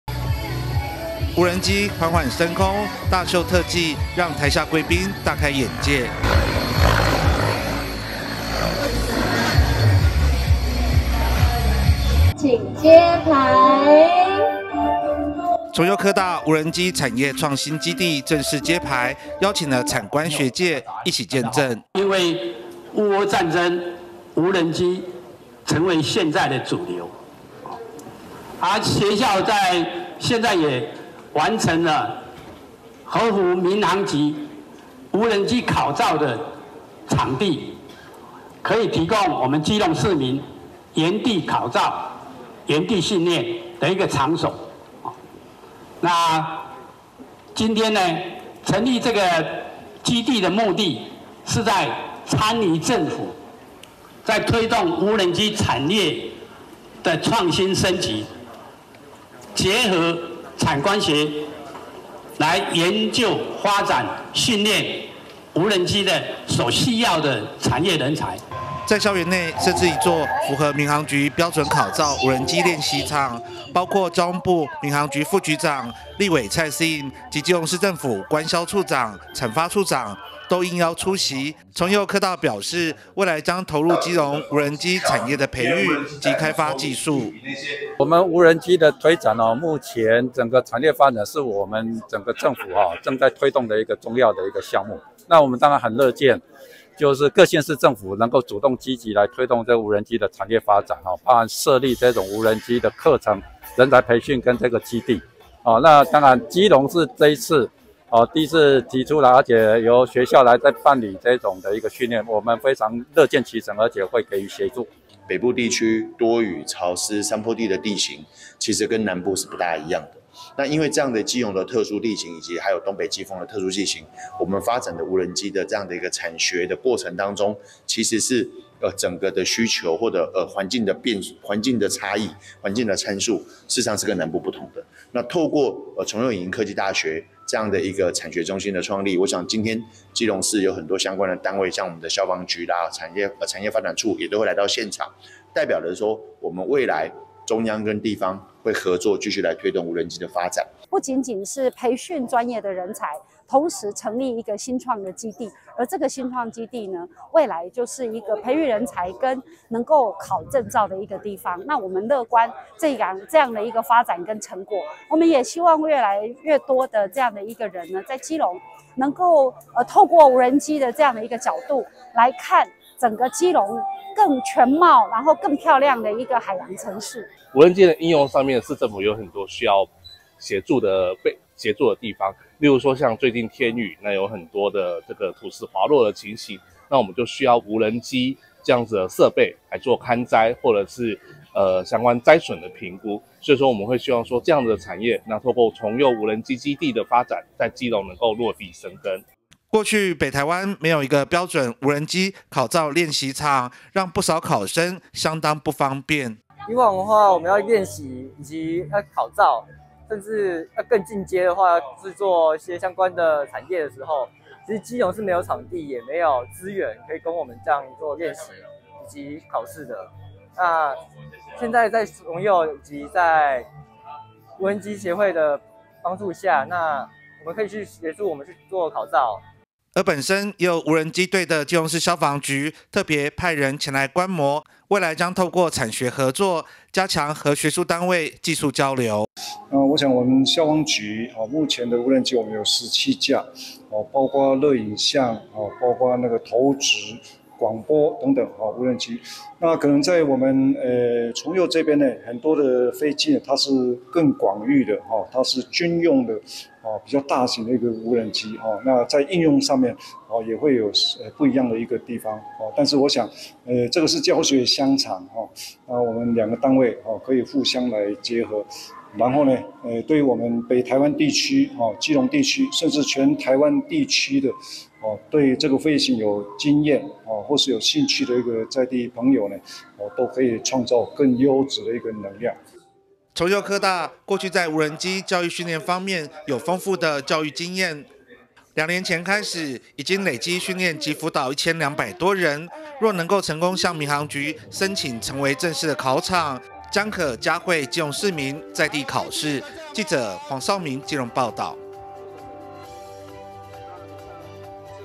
無人機｜ 崇右科大成立無人機產業基地 培訓英才
(中嘉新聞記者黃紹銘/基隆採訪報導) 基隆崇右影藝科技大學近年積極投入無人機教育，在校園內開設一座符合民航局考場標準的無人機練習場，成立「無人機產業創新基地」，產官學揭牌，將為基隆無人機產業培育英才及技術開發。